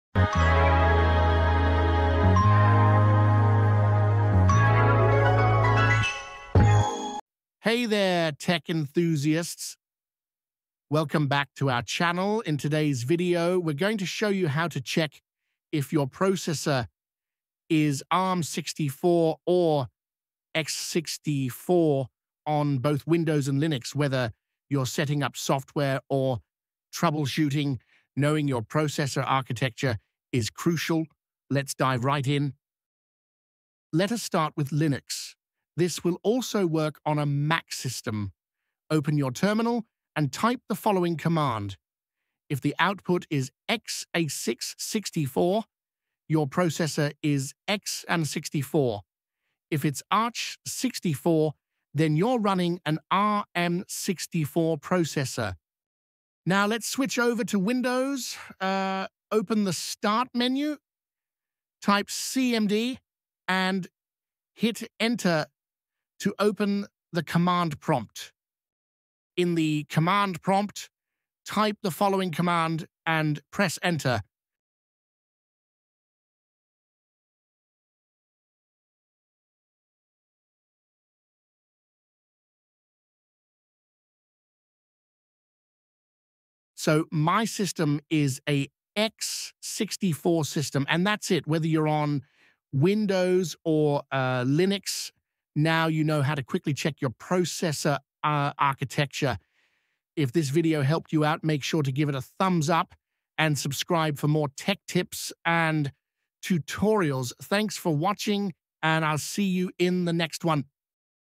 How to Check if Your Processor is x64 or ARM64 on Windows/Linux
Welcome to our latest tech tutorial! In this video, we'll guide you through the process of determining whether your processor is x64 or ARM64 on both Windows and Linux operating systems. Knowing your CPU architecture is crucial for installing compatible software and understanding your system’s capabilities.